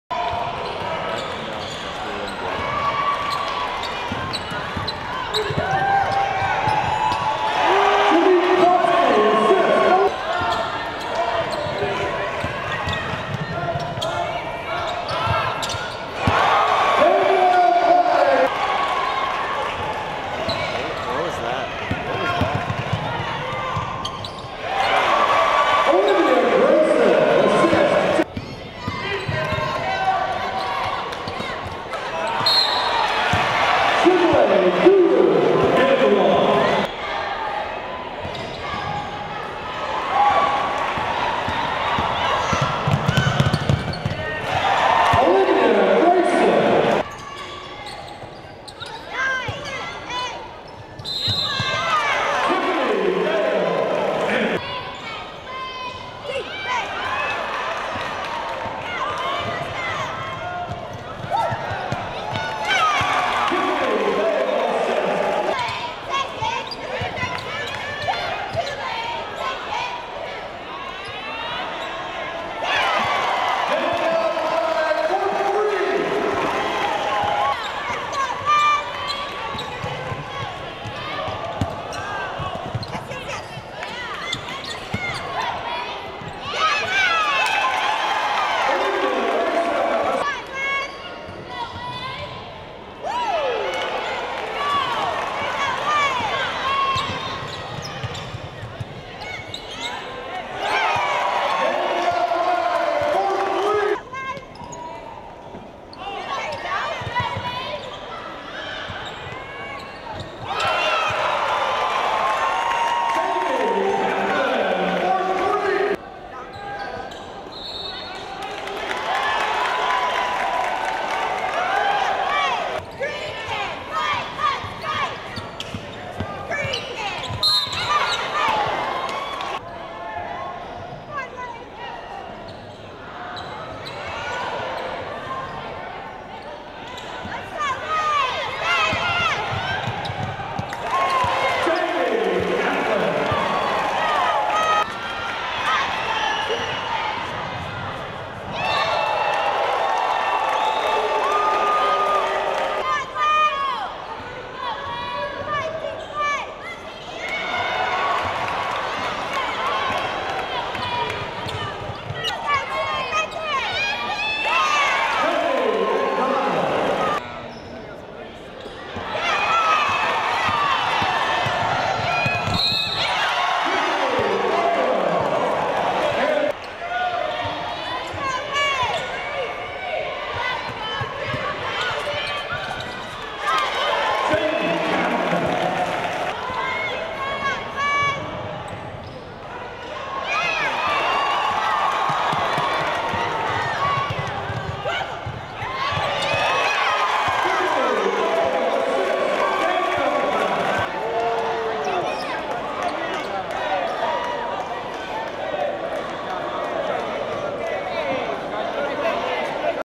Tulane Women's Basketball Highlights vs Memphis, 1/20/2013
Tulane defeats Memphis, 72-62